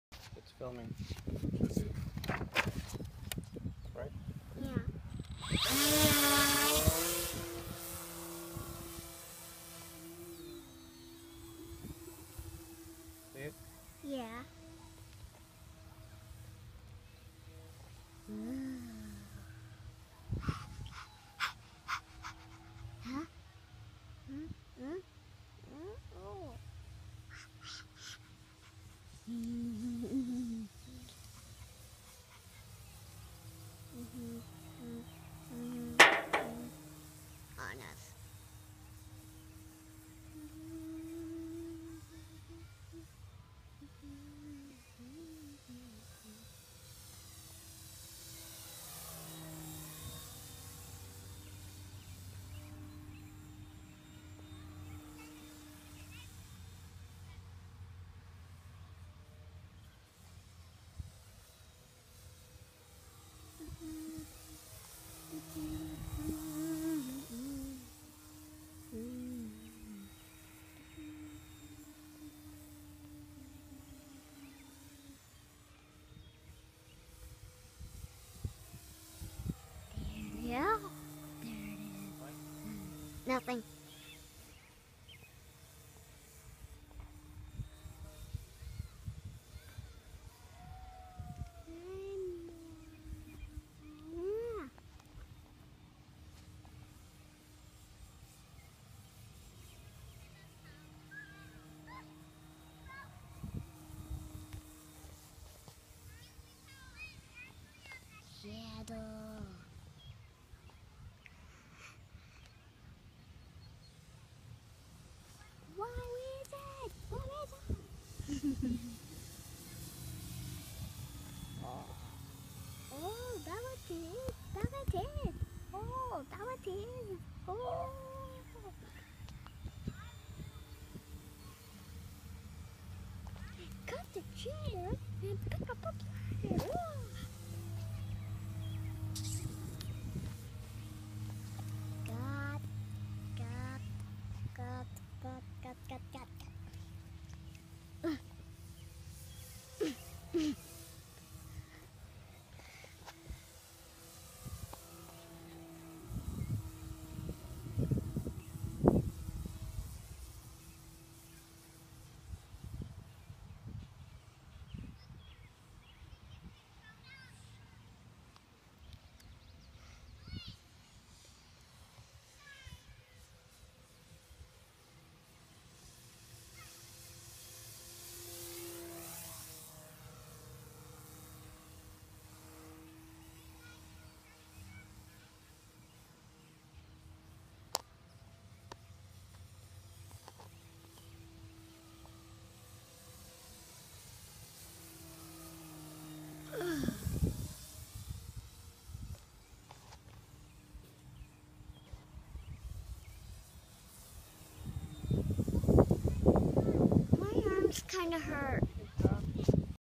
FT Flyer crazy flying weather
FT Flyer crazy flying weather.  Daughter is taping so bear with me here.